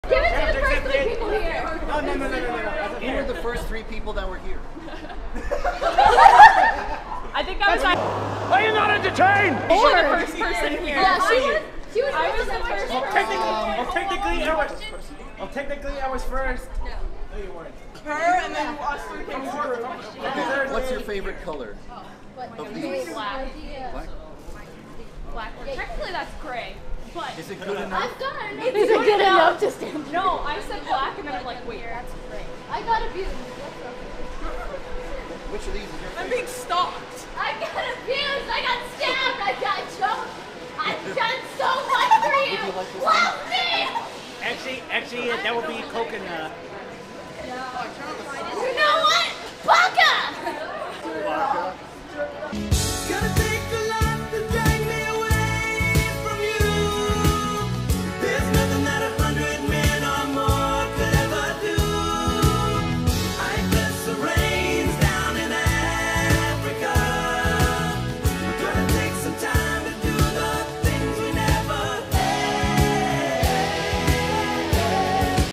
YandereDev Gives Shirts Only To Attractive Girls
The one senpai who was honest didn't get one.